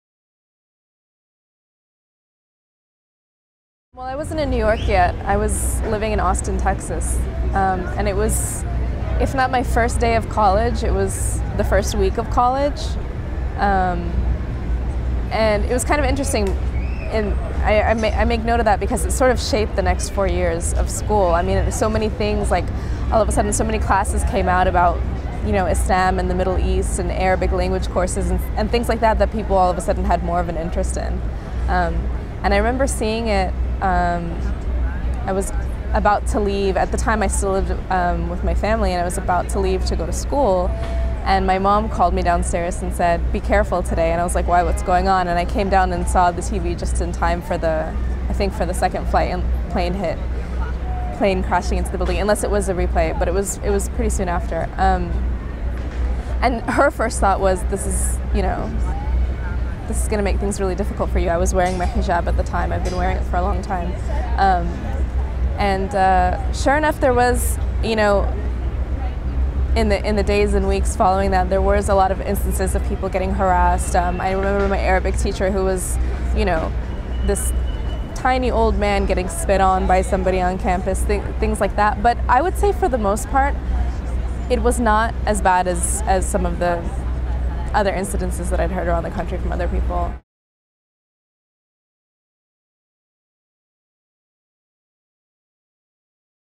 The 9/11 Decade: Sarah Abdurrahman on how 9/11 changed her life
Sarah Abdurrahman is a public radio producer in New York and a prominent Libyan tweeter who was active during the Arab Spring. Hear where she was on 9/11 and how those events shaped her life.